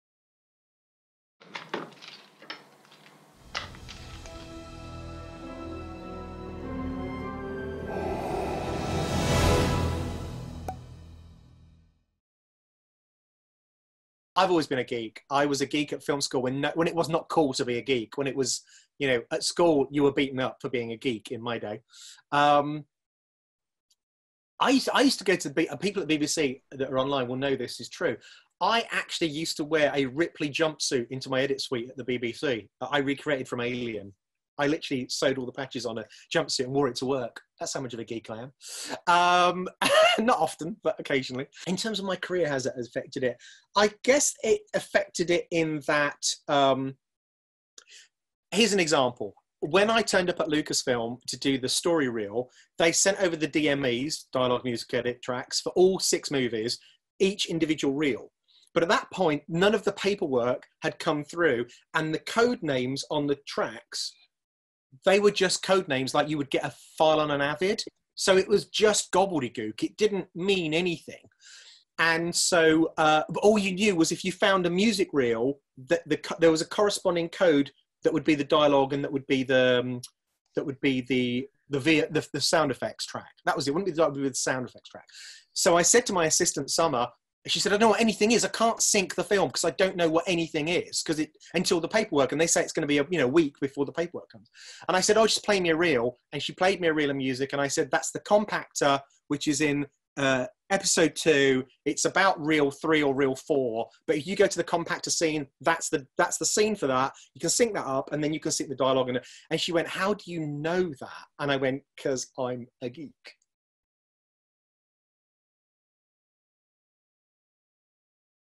Colin Goudie BFE on being a geek
A short excerpt from the ‘Virtual Glass of Wine’ British Film Editors interview series. Colin Goudie BFE talks about how being a geek helped him in his work.